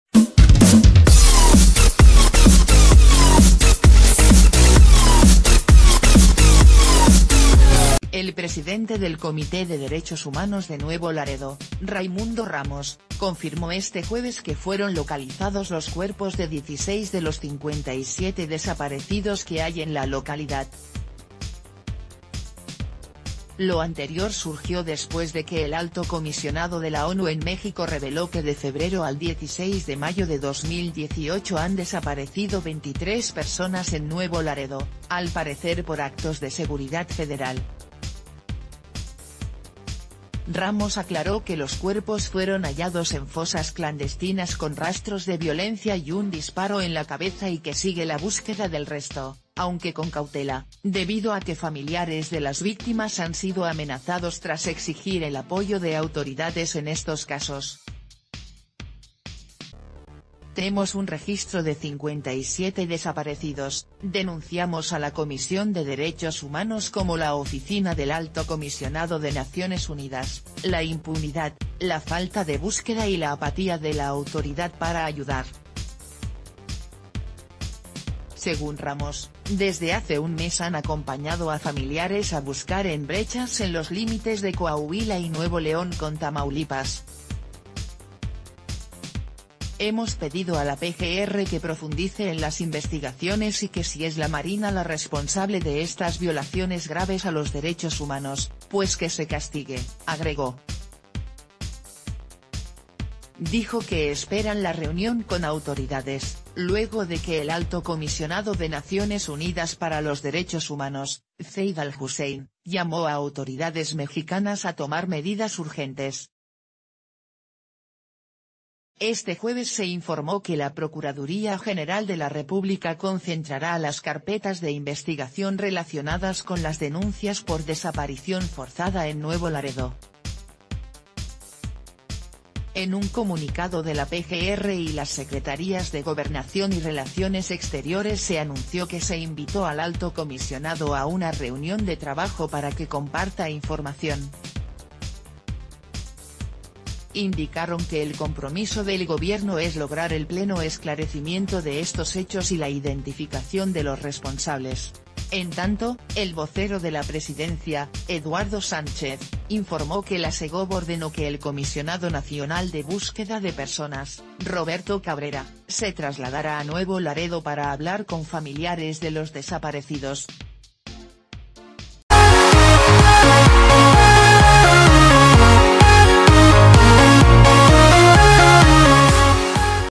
En Nuevo Laredo hallan 16 cuerpos de desaparecidos
El presidente del Comité de Derechos Humanos de Nuevo Laredo, Raymundo Ramos, confirmó este jueves q...